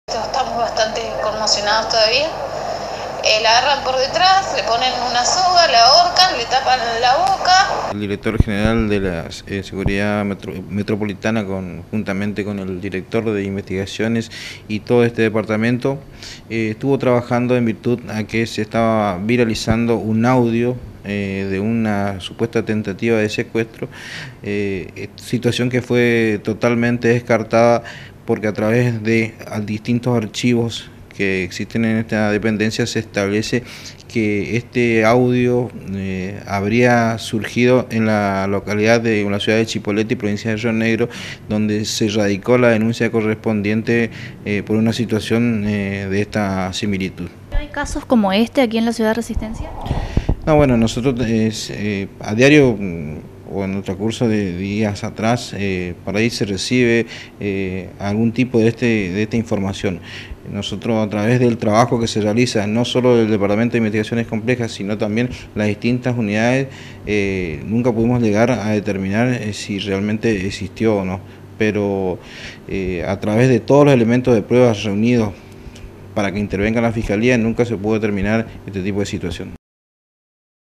C9 - Desmienten supuesto intento de secuestro
“NO FUE EN CHACO”
DESMIENTEN SUPUESTO INTENTO DE SECUESTRO DE UNA JOVEN EN RESISTENCIA
LA DENUNCIA SE RADICÓ EN LA PROVINCIA DE RIO NEGRO
ALDO MENDOZA